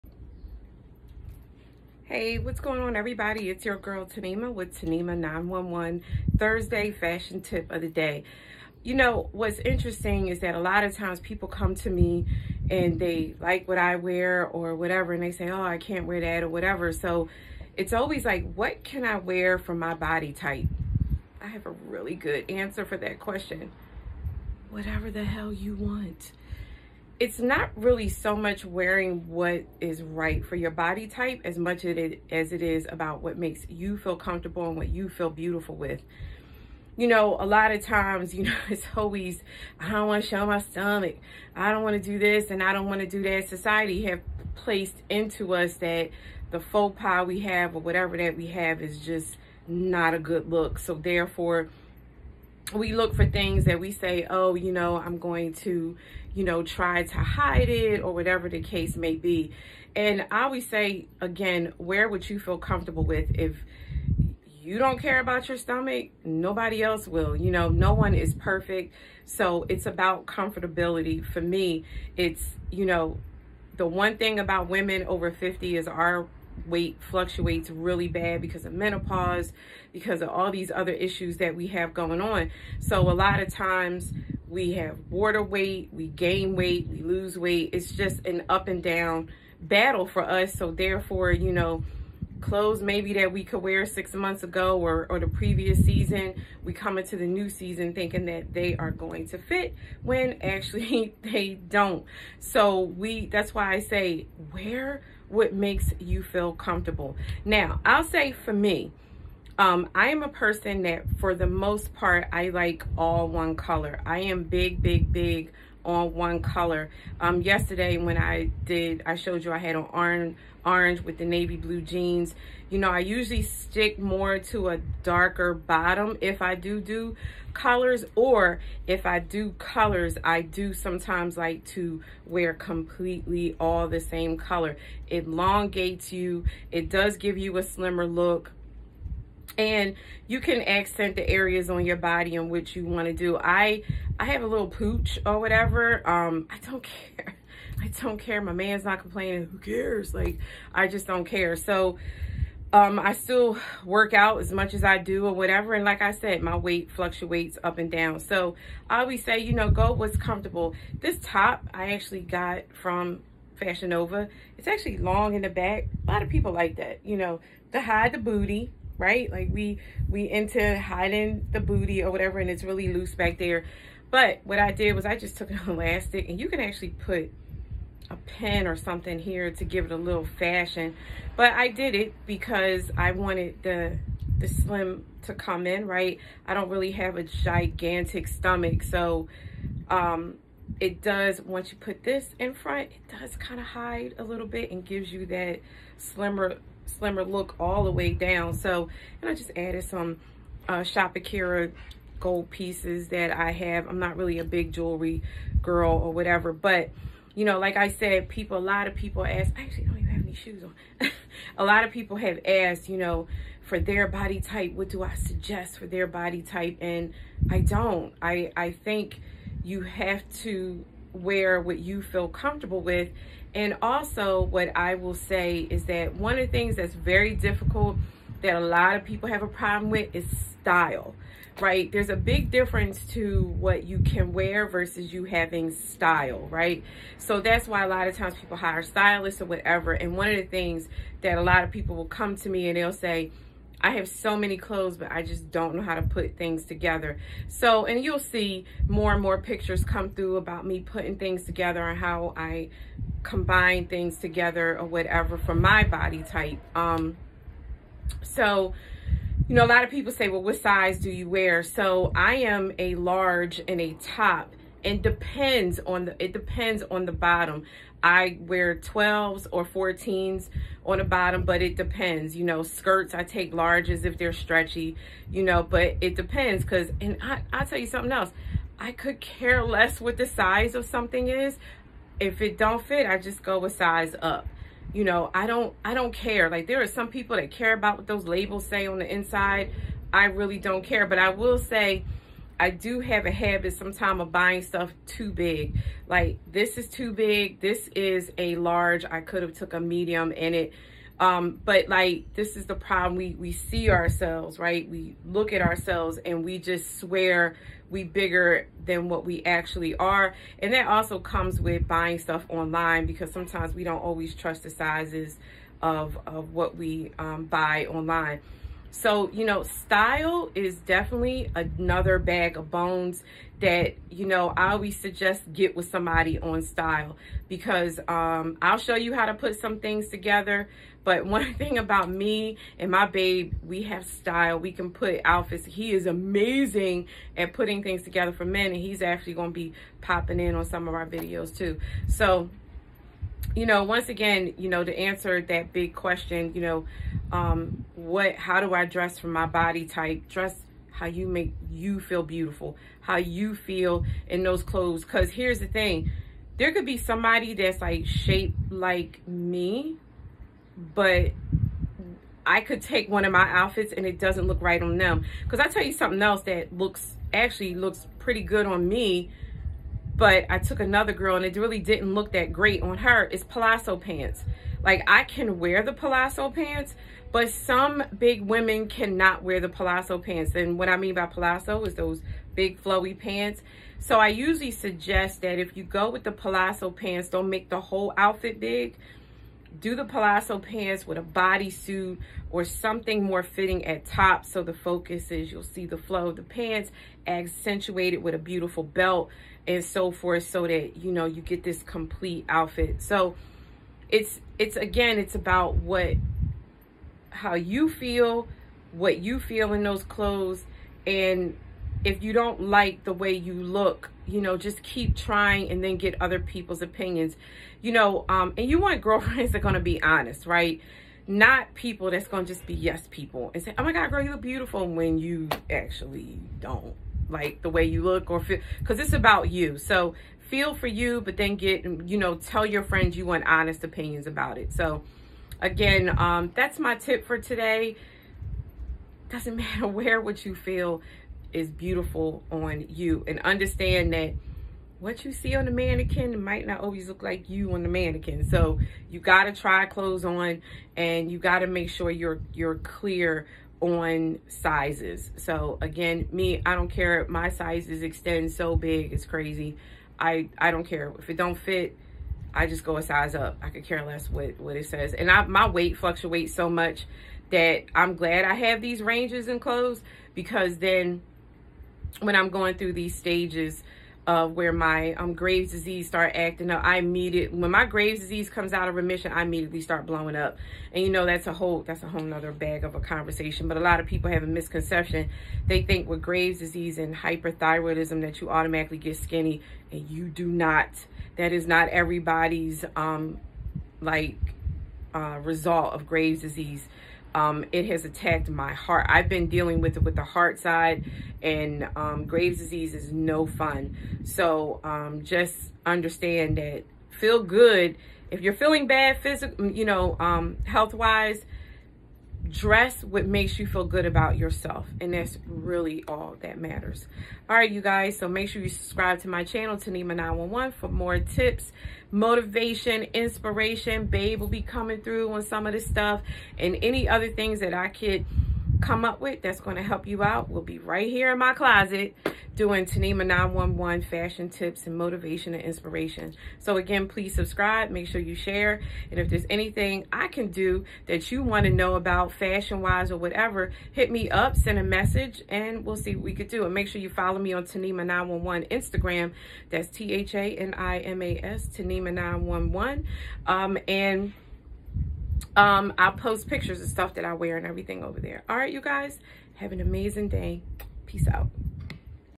Thursday fashion tip of the day . Body type and why it’s more about the mind then the shape
Mind over matter wear what the heck u want